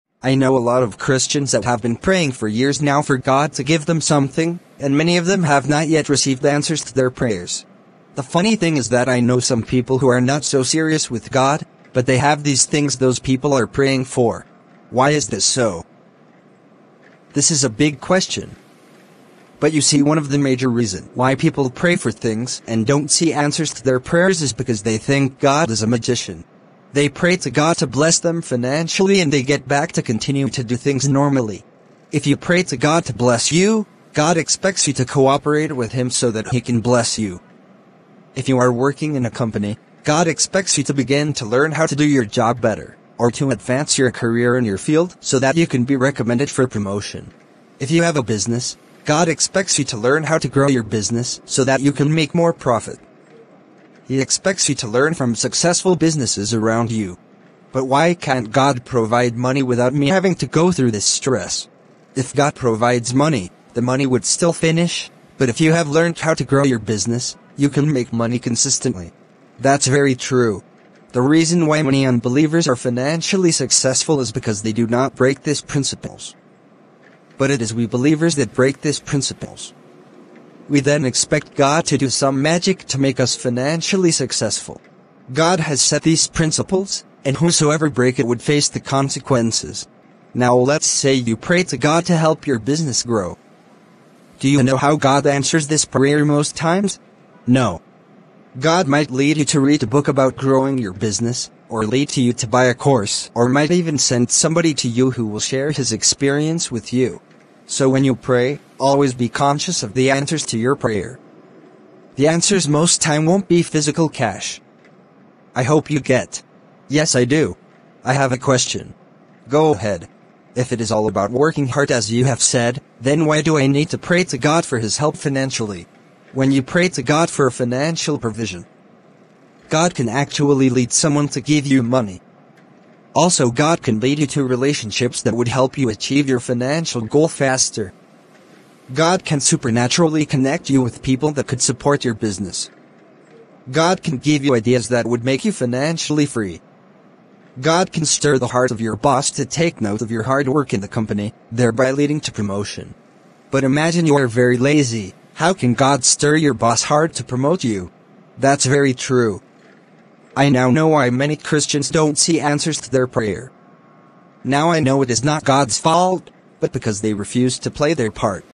Shocking Secret why unbelievers progress more than believers
Many times we expect God to do what we are supposed to do. If christians can take the things said in this video seriously, they would live a better life as a Christian. Many christians think God is a magician. They break earthly principles and expect to succeed. In this video I would be showing you why sinners have what many christians are praying for. Seat back and have a lovely time watching.